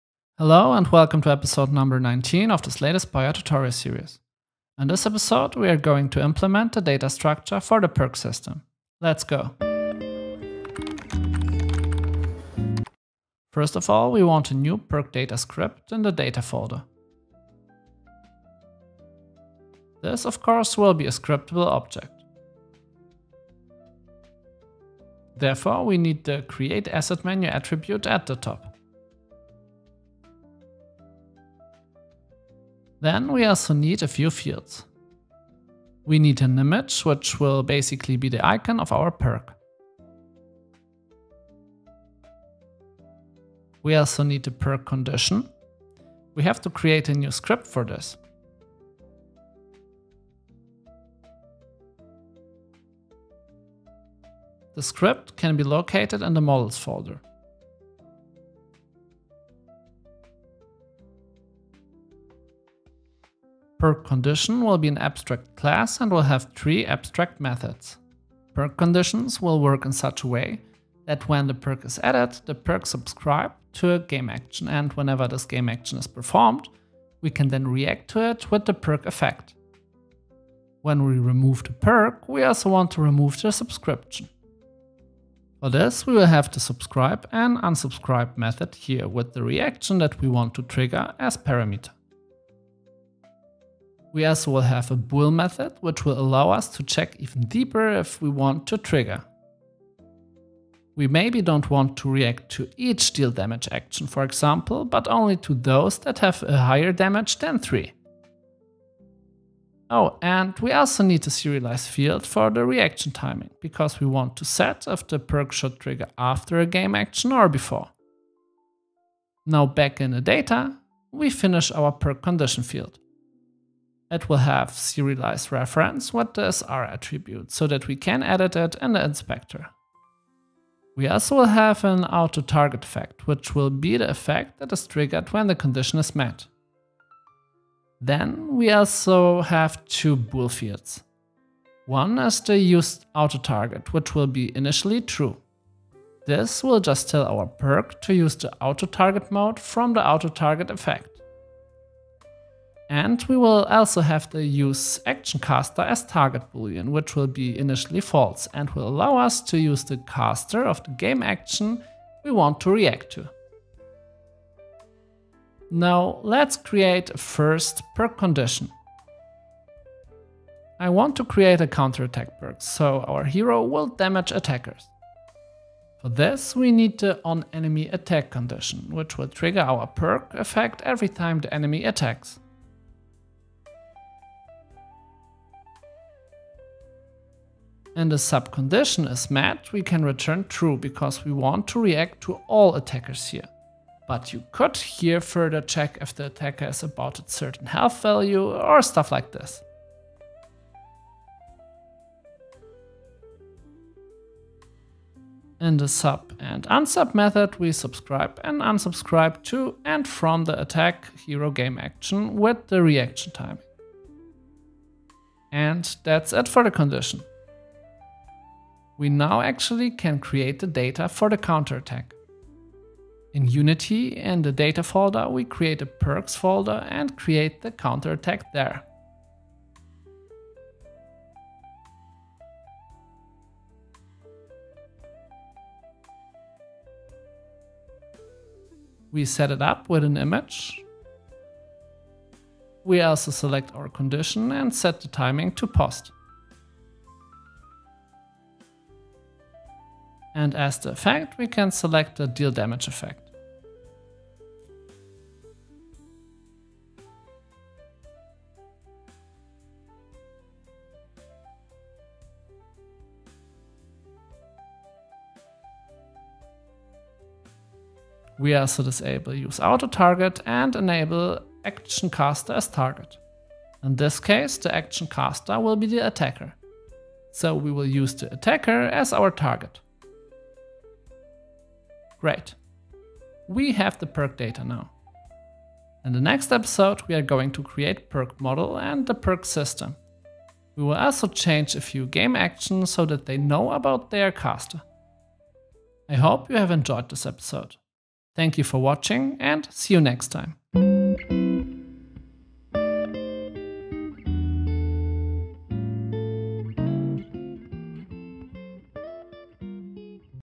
Slay The Spire in UNITY (Ep. 19) - Perk Data
A Slay The Spire Unity Tutorial Series where we will build the game from scratch. In this episode we create the perk data.